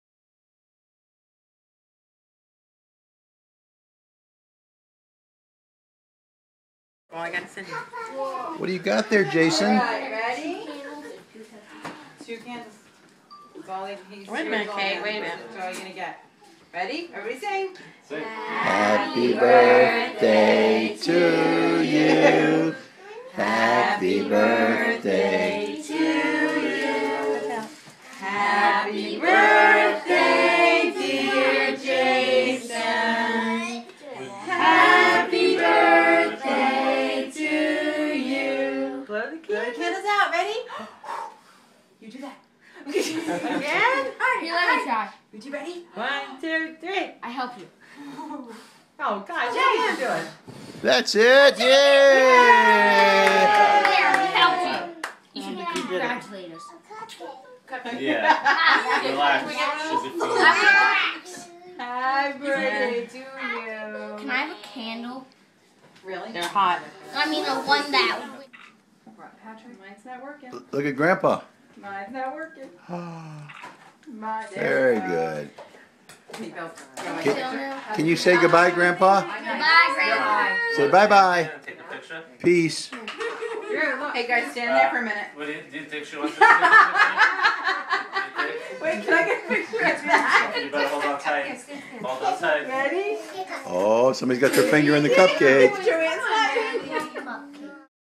Jason's2ndBirthdayPartymobile
Jason celebrates his second birthday with family and friends